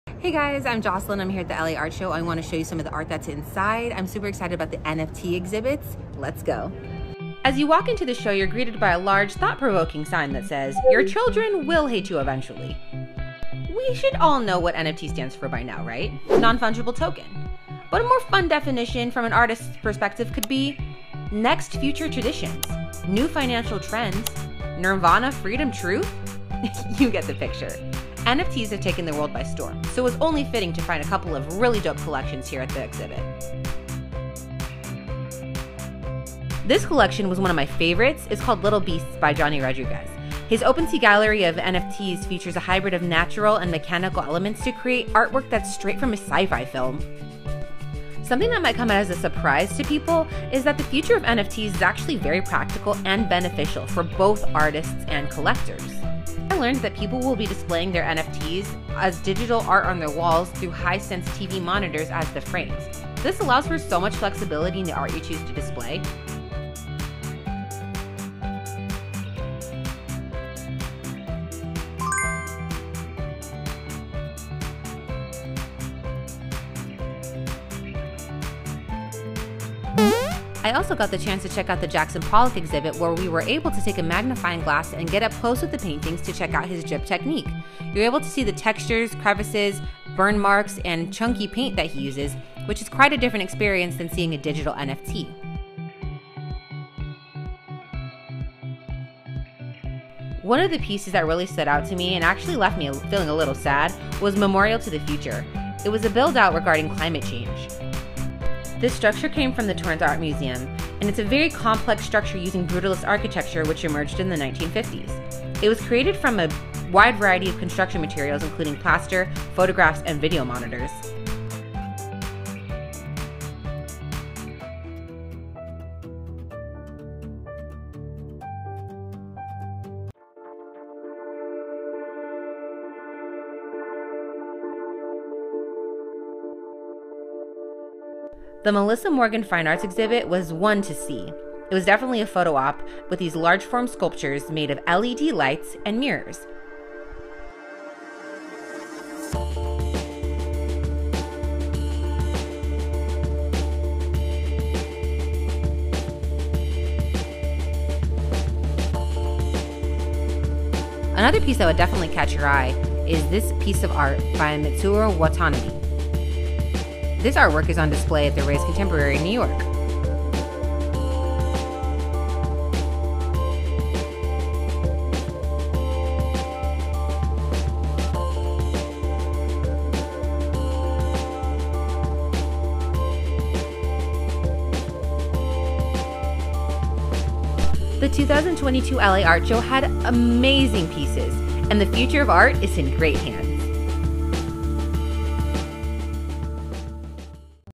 LA Art Show Walk Through 2022
I cover the exhibits that made the biggest impact at LA Art Show, 2022!
Which ones were your favorite?